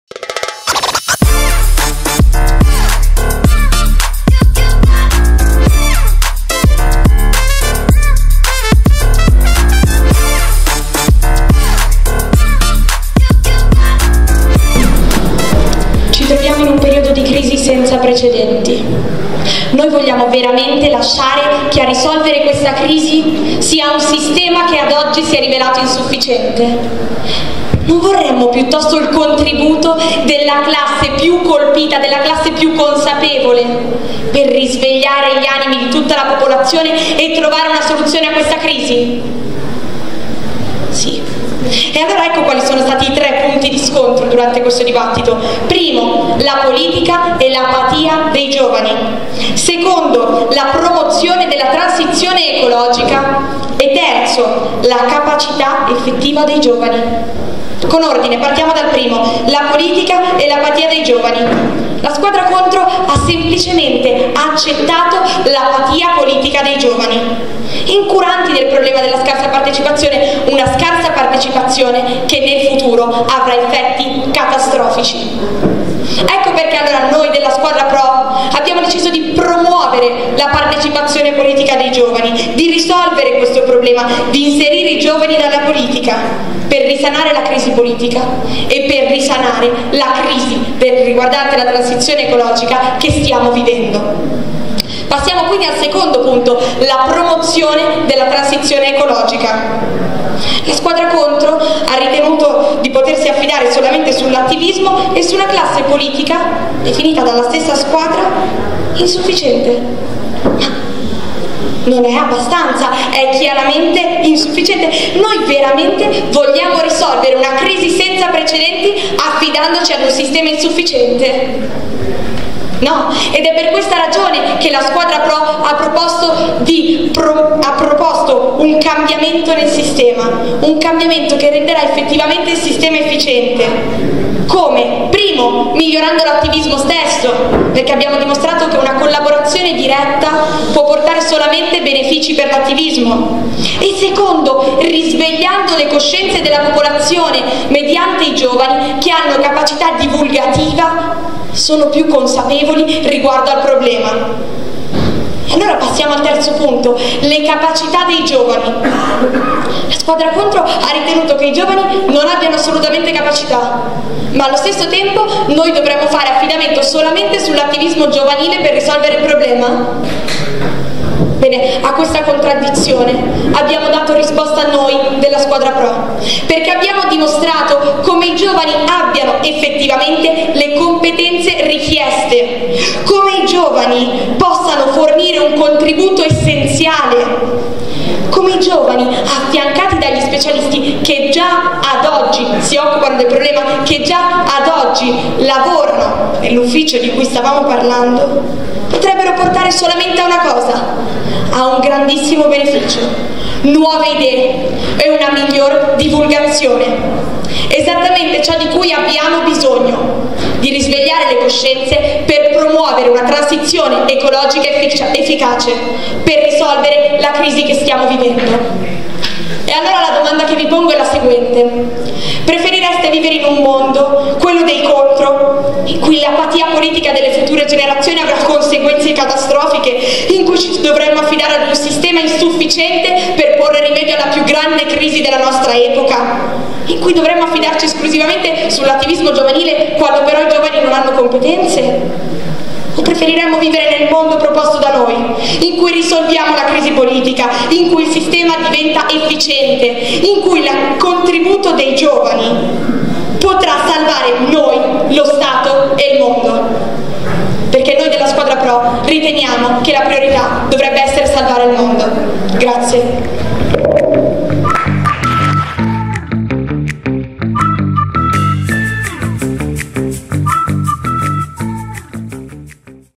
8.WSD. La replica della squadra PRO
Video tratto dalla finale dei Campionati Nazionali di Debate (edizione del 2022) organizzati in collaborazione con la rete WeDebate finale Lombardia VS Emilia Romagna Olimpiadi Nazionali Debate 2022 Sez Italiano